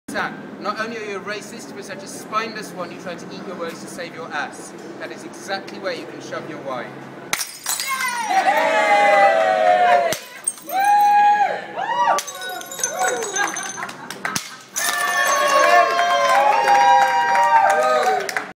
What to do with racist wine
Jacob Kenedy (Bocca di Lupo), in 2013, shows racist Italian politician Fulvio Bressan what to do with his wine.